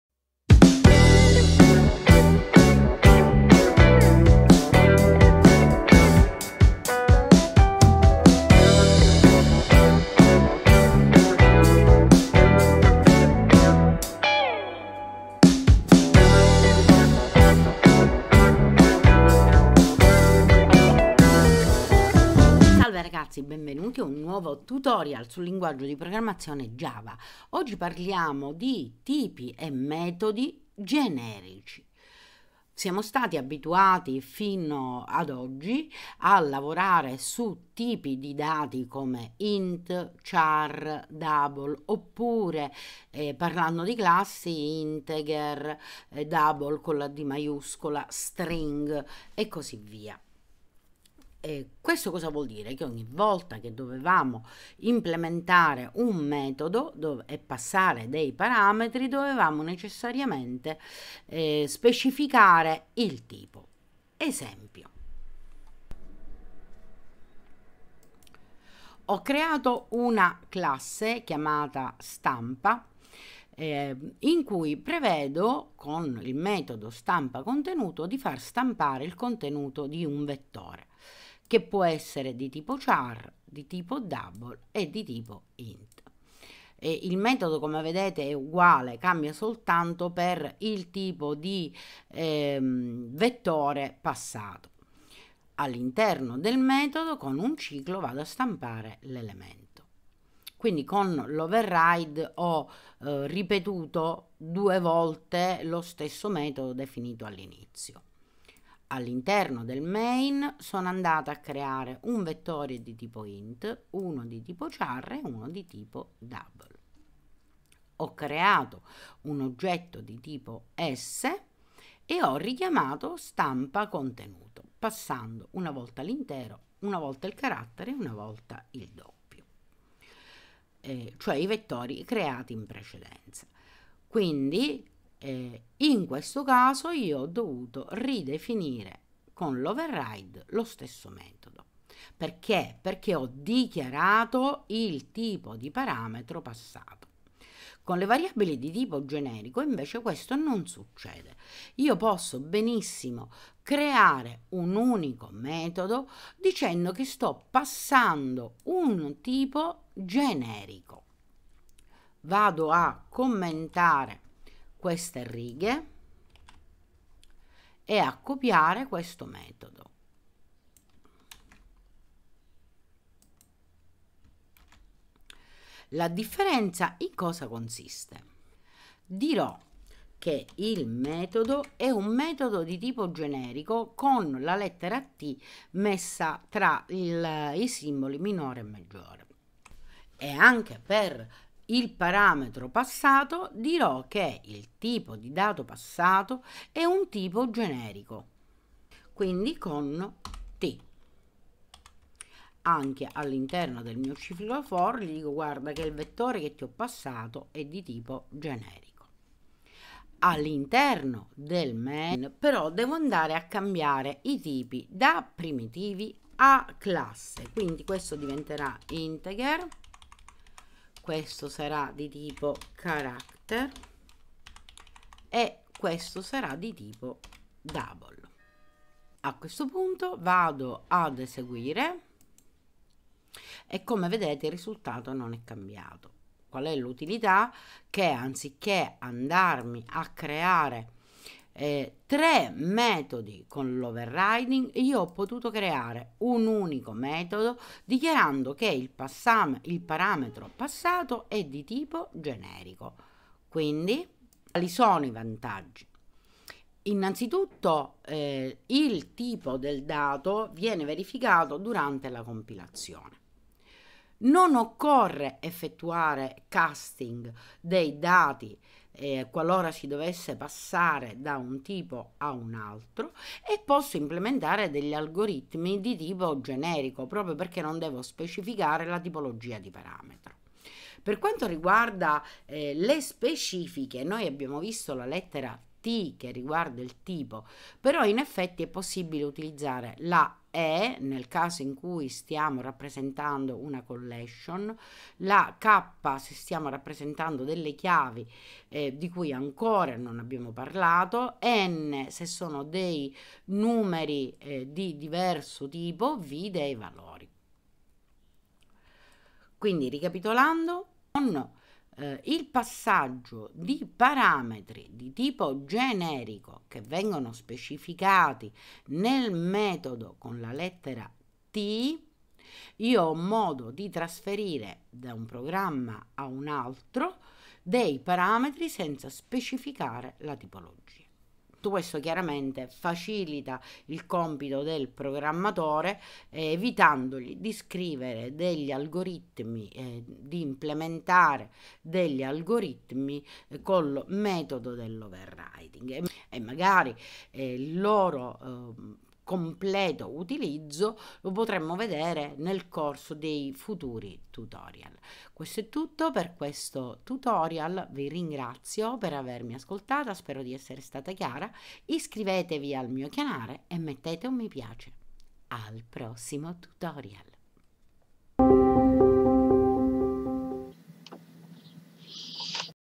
Java #50 Tipi e metodi generici
A cosa servono i metodi generici? Esercizio sulla stampa di 3 vettori di tipo diverso con il passaggio di un parametro generico